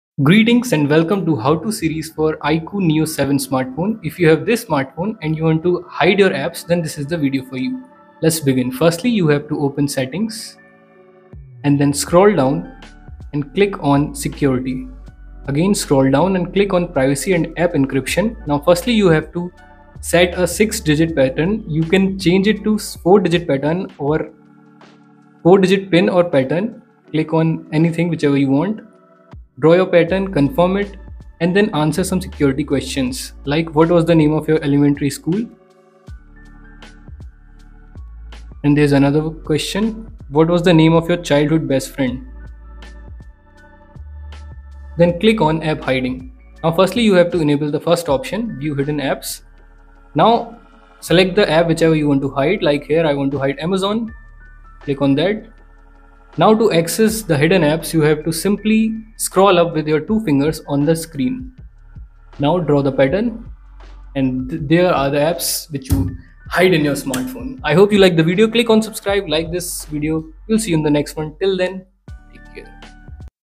How to Hide Apps in IQOO Neo 7 Pro/Neo 7|English tutorial
Welcome to our step-by-step guide on how to hide apps on your IQOO Neo 7 Pro or Neo 7 smartphone!  Privacy matters, and we're here to help you keep your personal apps discreetly tucked away from prying eyes.

In this tutorial, we'll walk you through the easy process of hiding apps on your IQOO Neo 7 Pro/Neo 7 device. Whether you want to keep sensitive apps private or declutter your home screen, we've got you covered.

🔍 What You'll Learn:
- Quick and simple steps to hide apps
- how to access the hidden apps
-how to unhide the hidden apps

Don't compromise your privacy – watch the video now and master the art of hiding apps on your IQOO Neo 7 Pro/Neo 7 smartphone.

Got questions or need help? Drop them in the comments below. 🙌